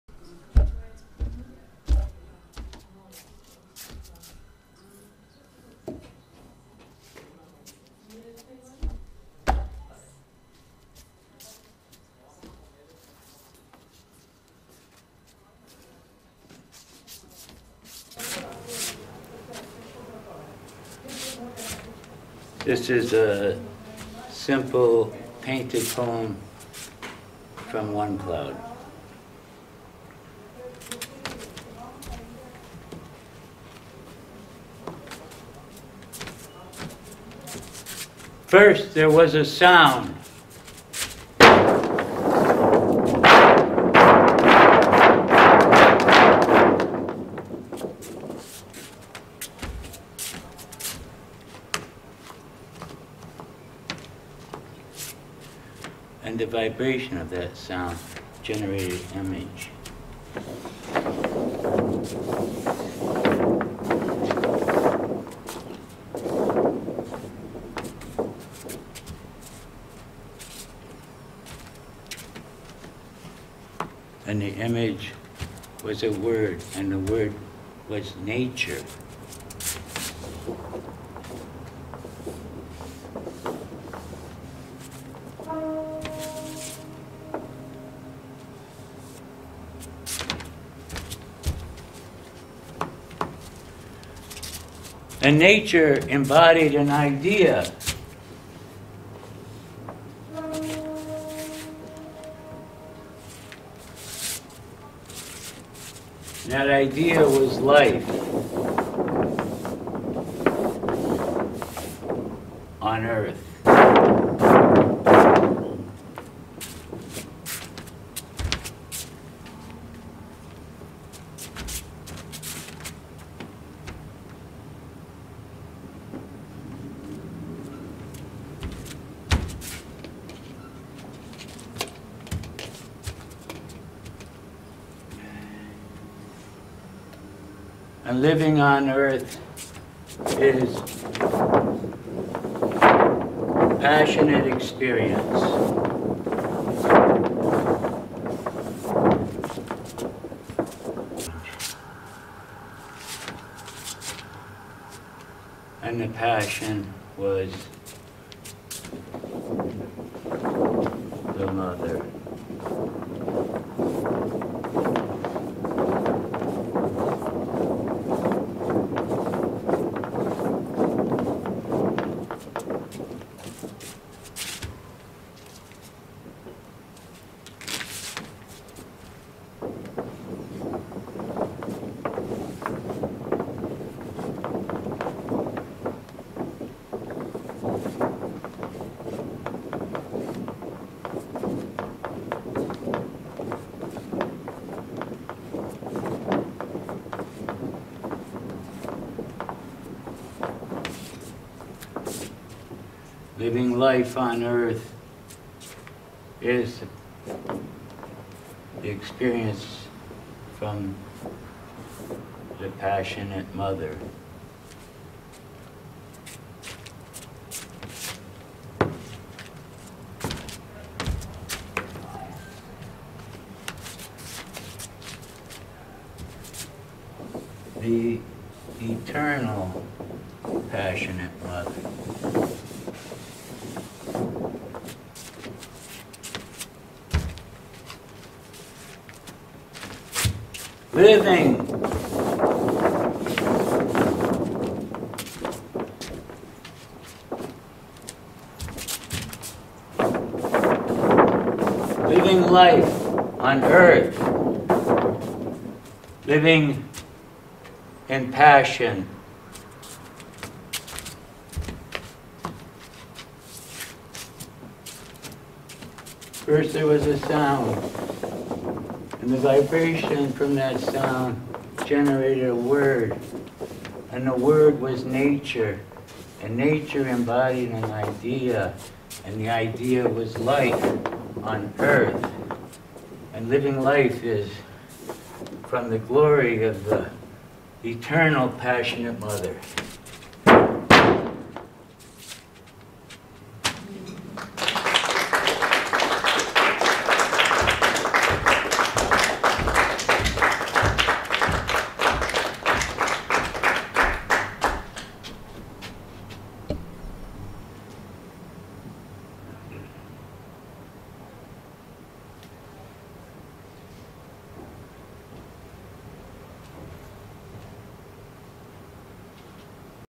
onecloud paints the eternal mother creation Aug.13, 2022
onecloud painted poem at Arcadia Housing Co-Operative
Aug. 13, 2022
video by marty smith
read marty's publication, 'a passionate creation'

i now offer a paid subscriber option, if you’d like, please consider an upgrade. Your support means the world.